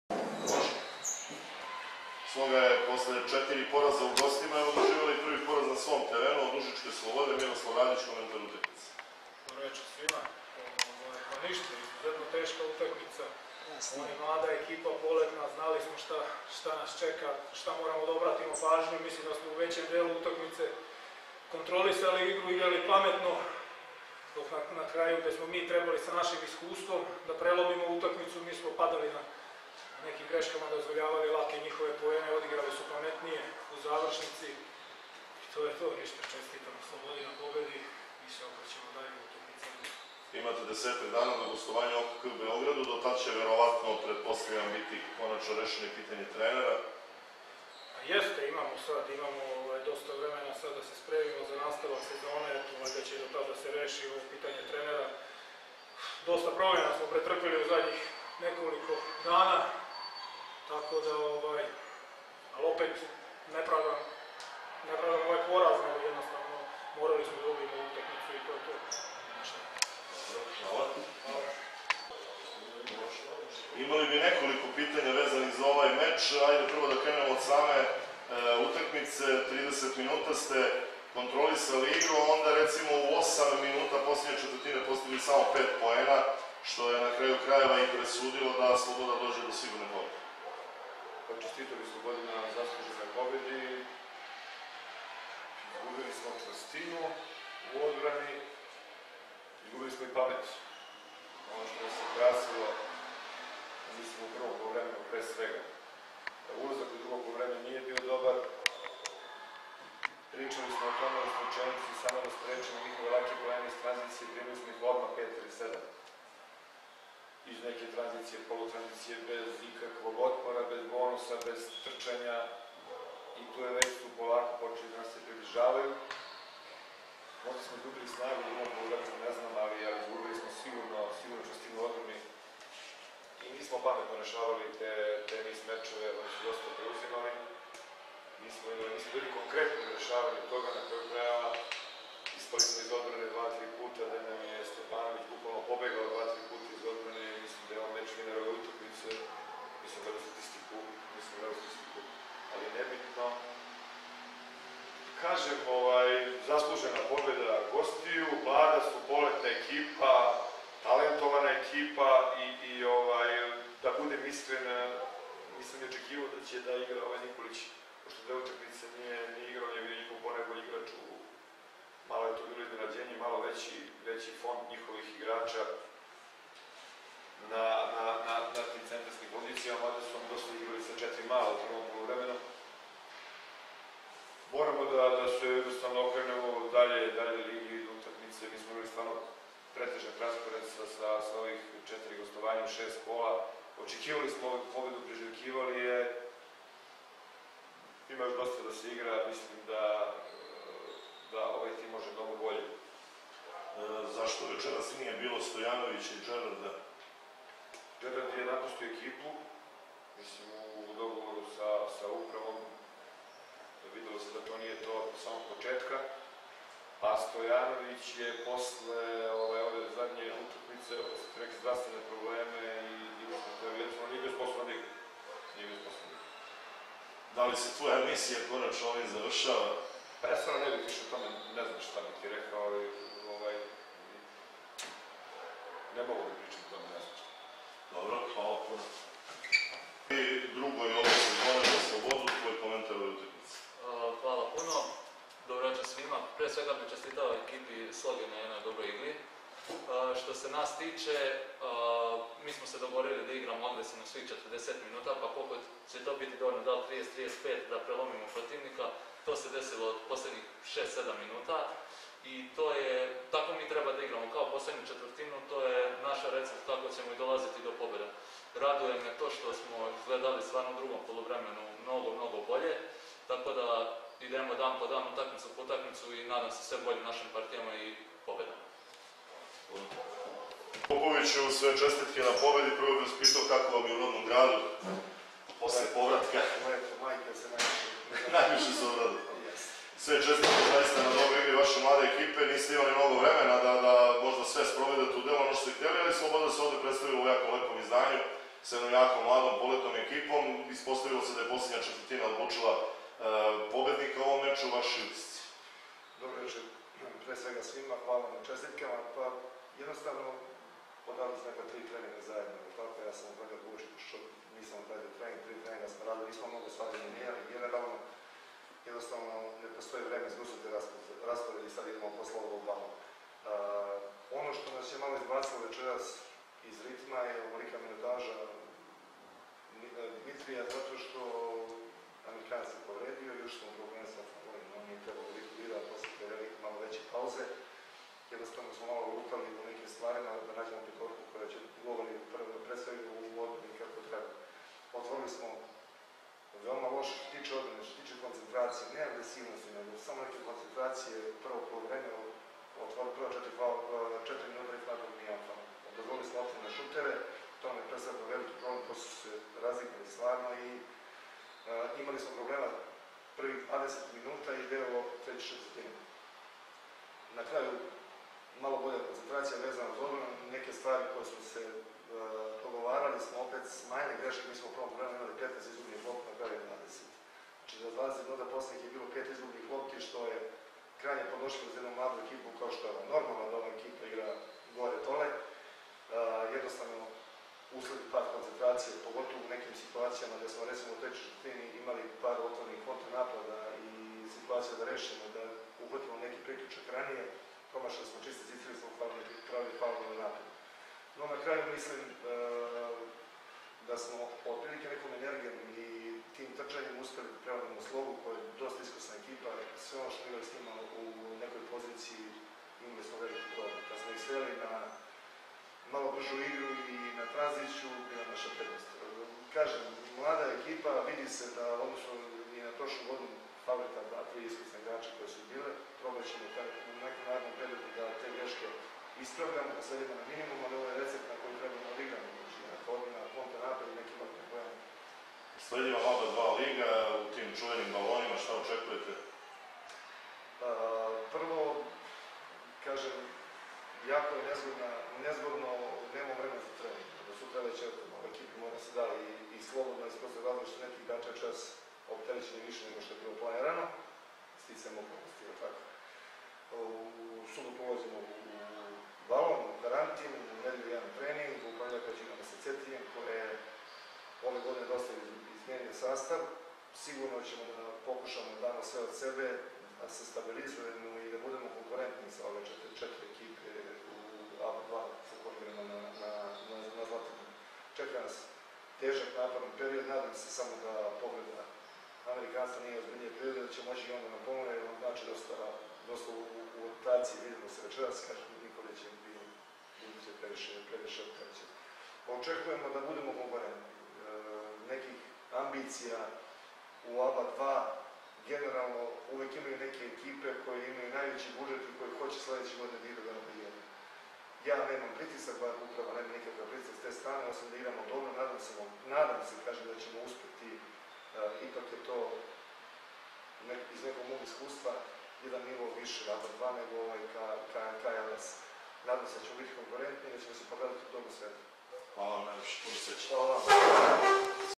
Press konferencija KK Sloga KK Sloboda (67:73) 7.kolo KLS 2020/21
Utisci sa pressa nakon završenog susreta KK Sloga KK Sloboda (67:73) 7.kolo KLS 2020/21 Izjave aktera susreta igrača i trenera obe ekipe: Miroslav Radić Stefan Simić Oliver Popović